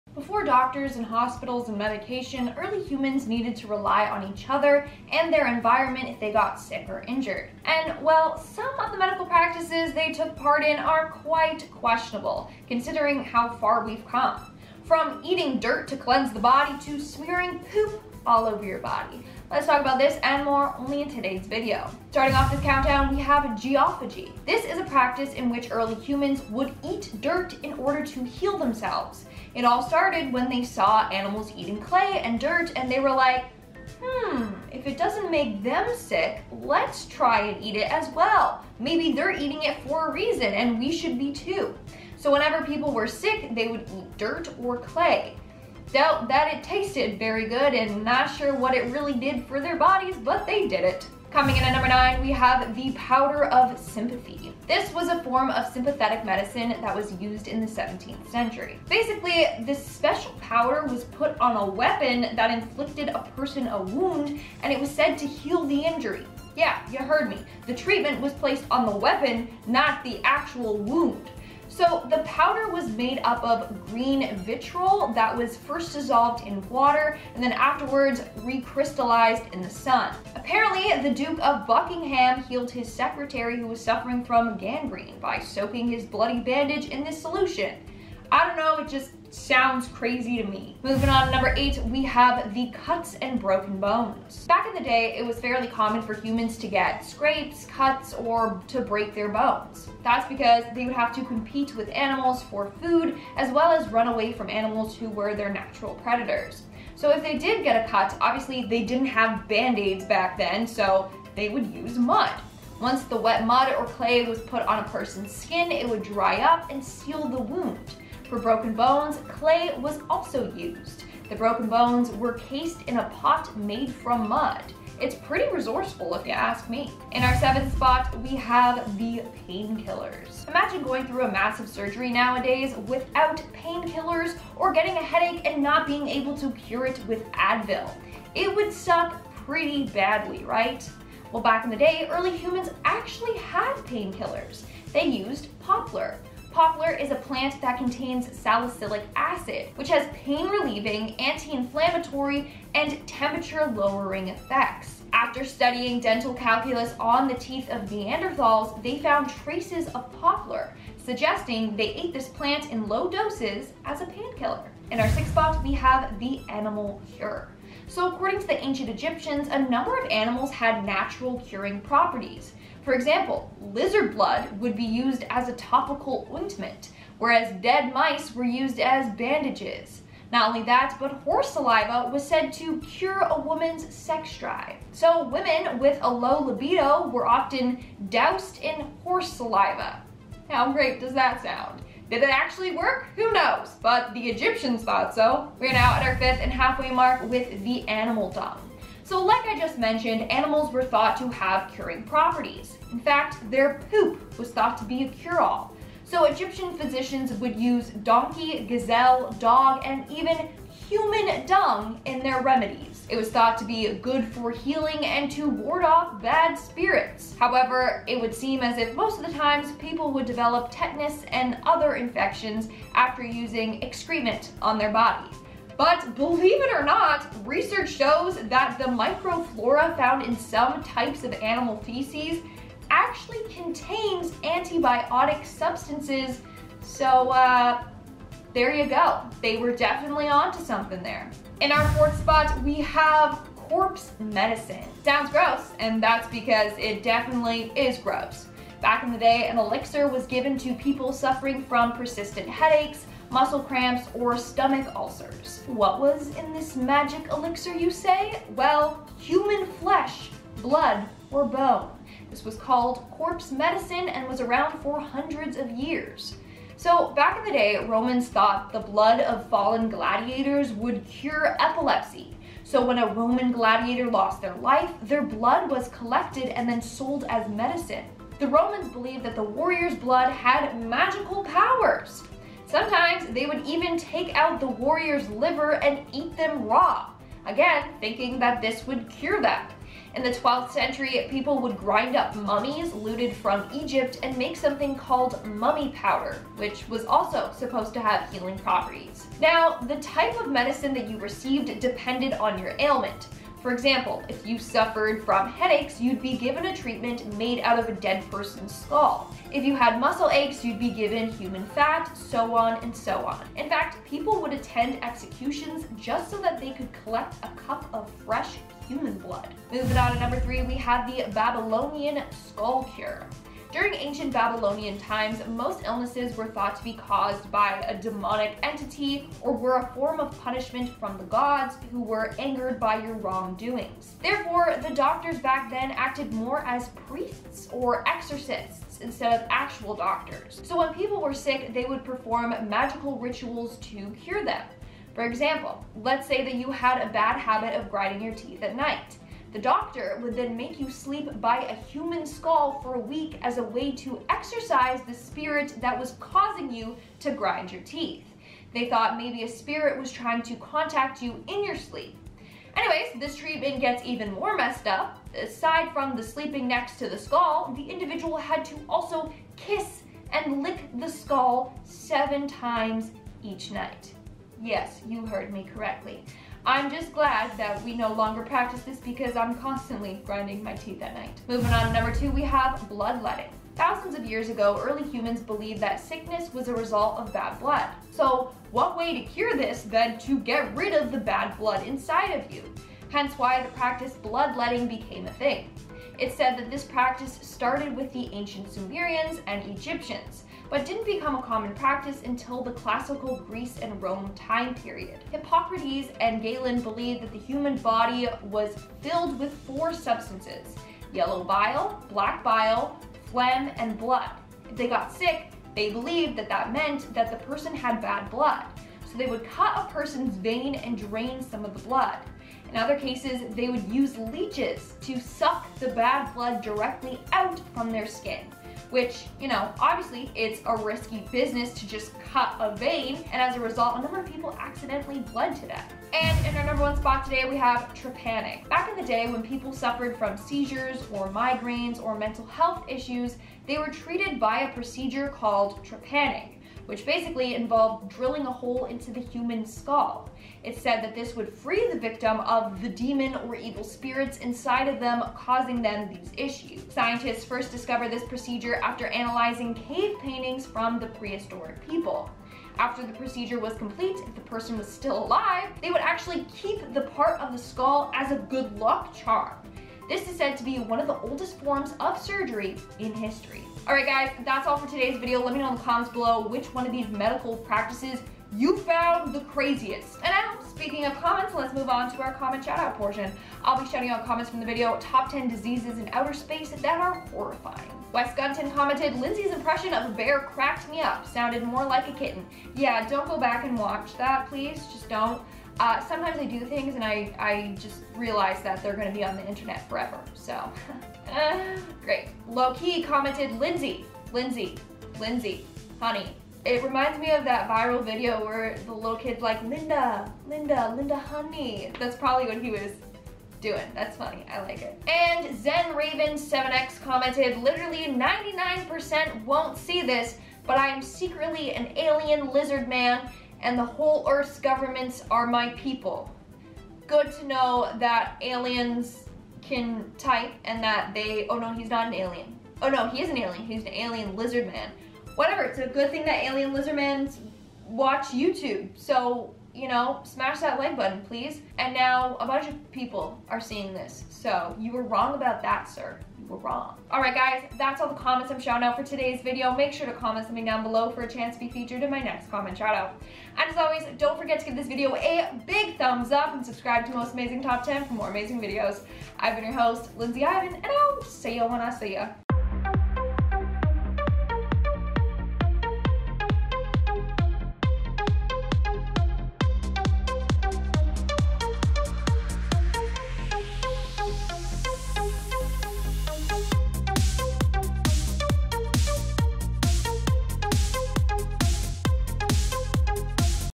Top 10 Medical Practices Early Humans Performed
Ancient medicine practices were really creative and at times terrifying. Today we shall take a look at the history of medicine and some of the most profound and unique treatments. This is the Top 10 Medical Practices Early Humans Performed